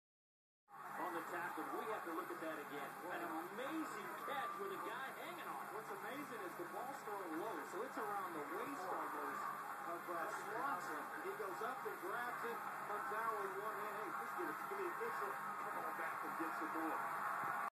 Montana State player makes amazing catch
Montana state player makes outstanding catch in Semi final game versus Sam Houston